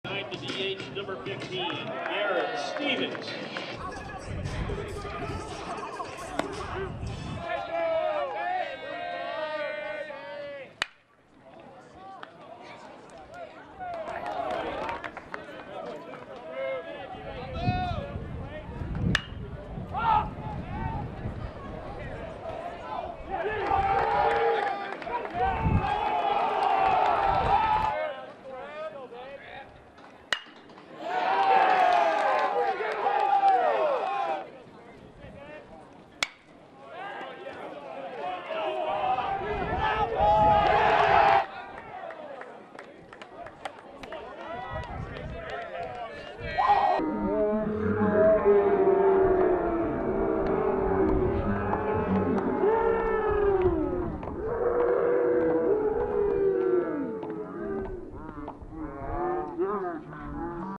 BSB SBU V.S. MSSU (4/1/2016)
College Baseball highlights shot by me.